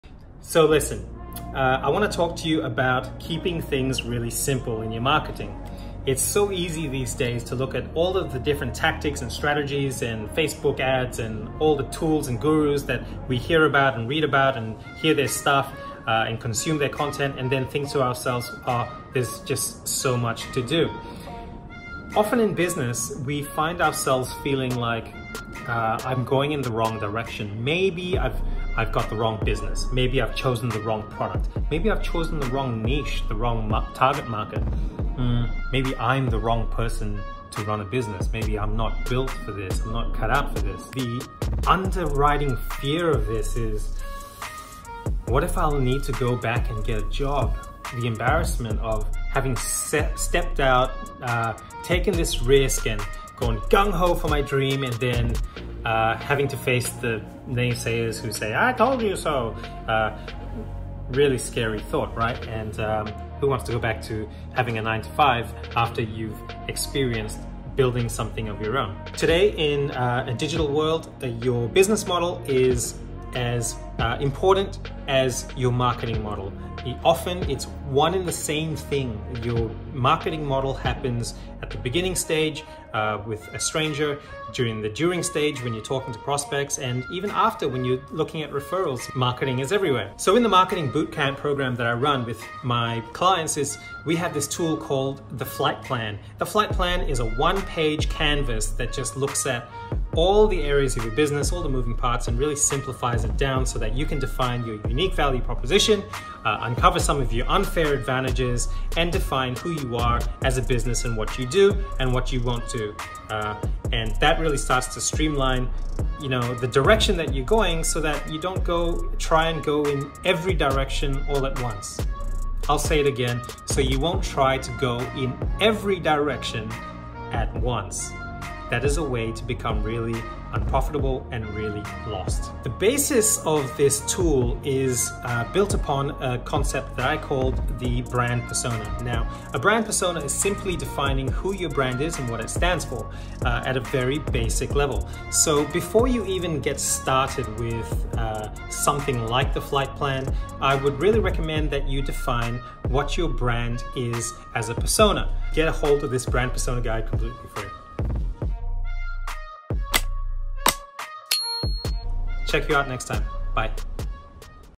Why You Should Approach Marketing Like Your Accountant
Not knowing your metrics is a common small business mistake where you the owner of the business don't know how many strangers are turning into customers. This leads to unprofitable marketing, and at that point you hit a brick wall and can't find a way forward. This video highlights the symptoms and some of the causes.